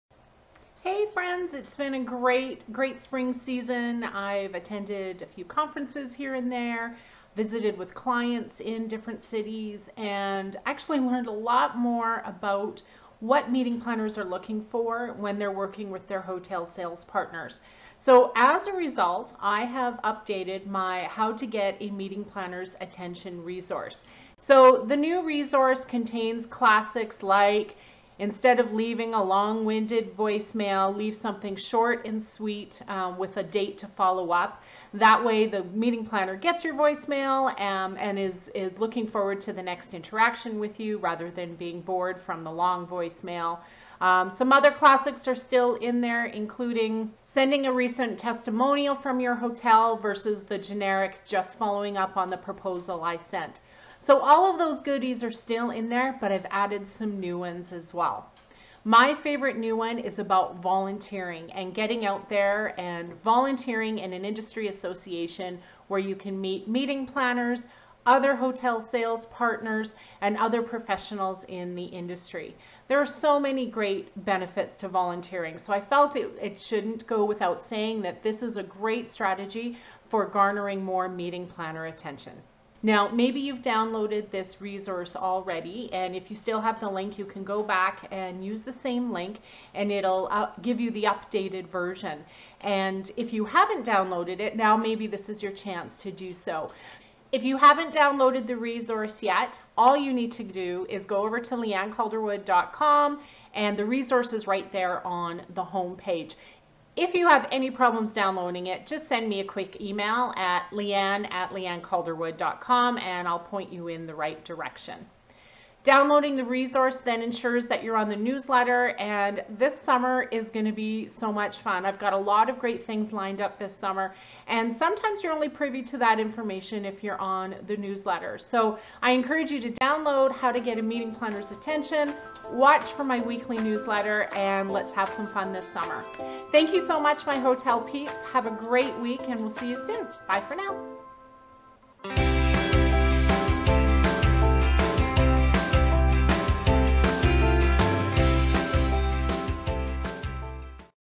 How to Get A Meeting Planner's Attention
Do you work at a hotel or destination marketing organization?  Do you service meeting planners in their quest to craft and execute the perfect meeting?

If you help meeting planners with their events, this worksheet is for YOU!  I've outlined some tips on how to get their attention, and make your service (and your personal brand) stand out against all the others.